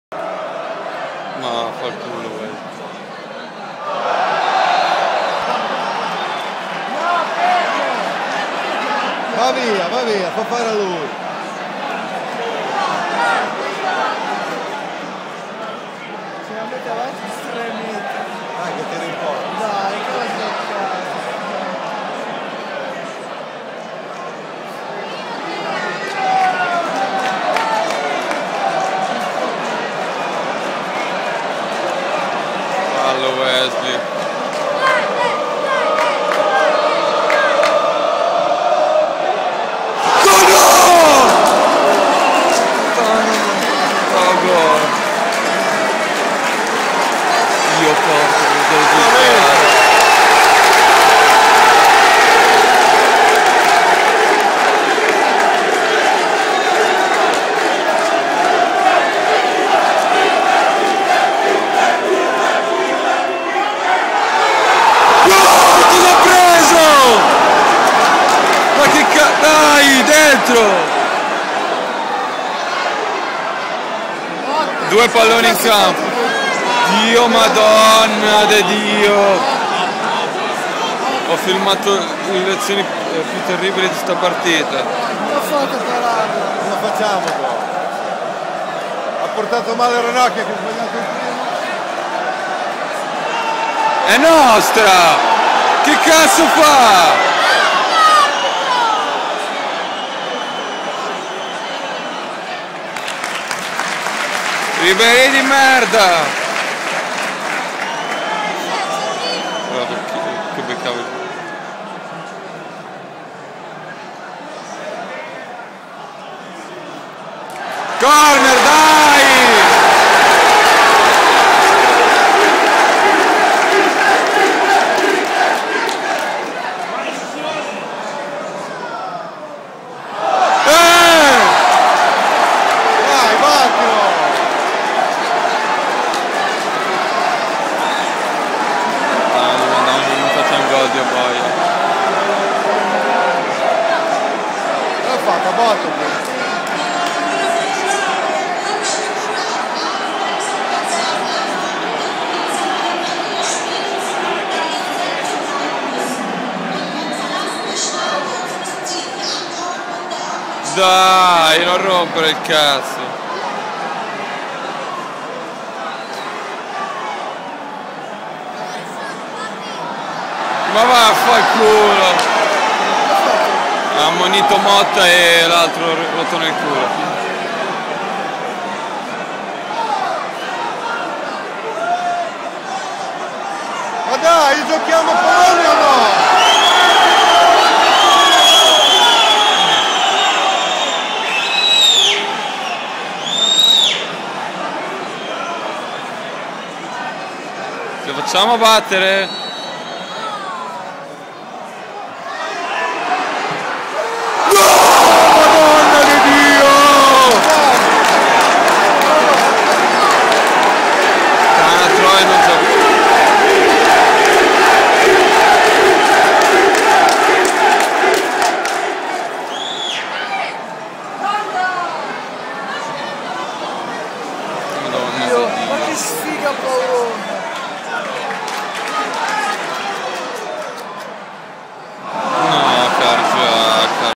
Inter-Bayern Munchen clamorose palle gol!
Punizione di Wesley che da l'illusione del gol, parata strepitosa su T.Motta e tiro di Eto'o deviato fuori di nulla...